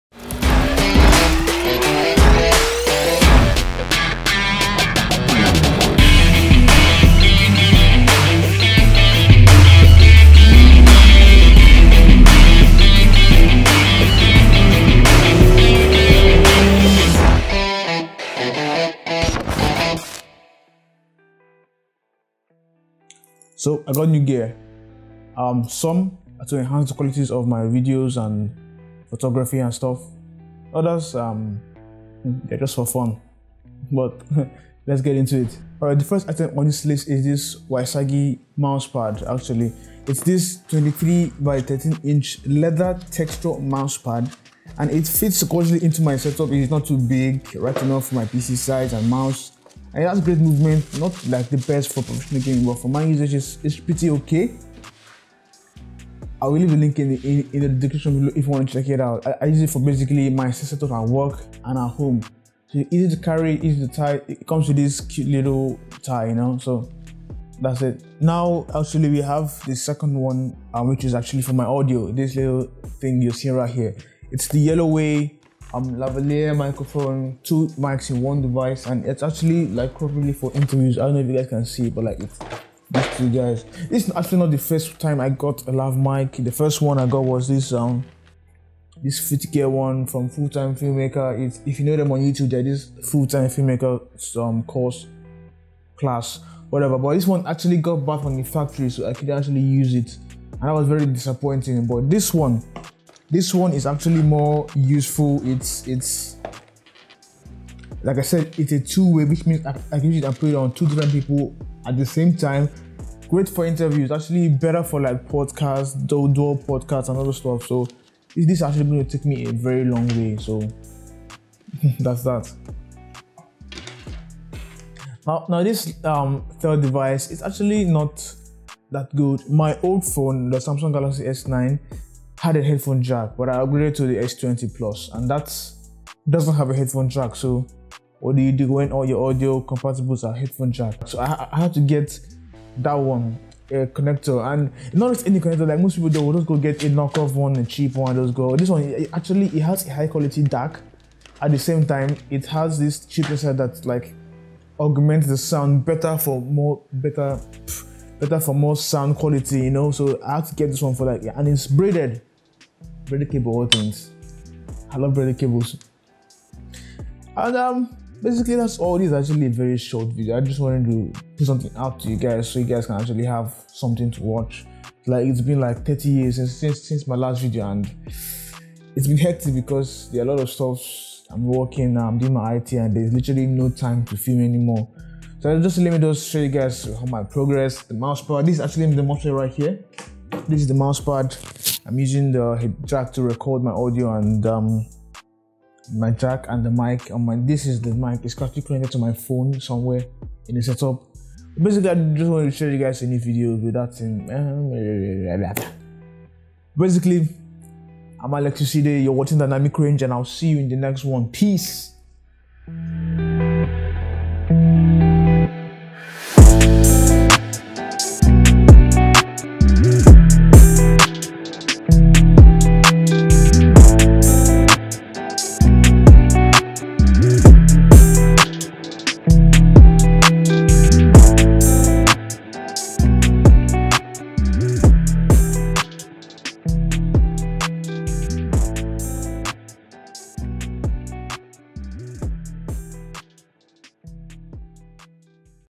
NEW GEAR!!! (New Tech for the Setup)
So I know it's been quite a while since my last video, i've been caught up with work and other stuff, but during my absence, I did acquire a few new tech for the setup and for personal useeeeee.
So this is jsut a video for you guys to enjoy my face againg hehehe 😂😂😂, and if you would like to add this to your setup or get some for yourself, then this video is for you...

Ysagi non-Slip Desk pad

YelloWay Dual Lavalier Lapel Microphone

Jsaux USB Type C to 3.5mm Female Headphone Jack Adapter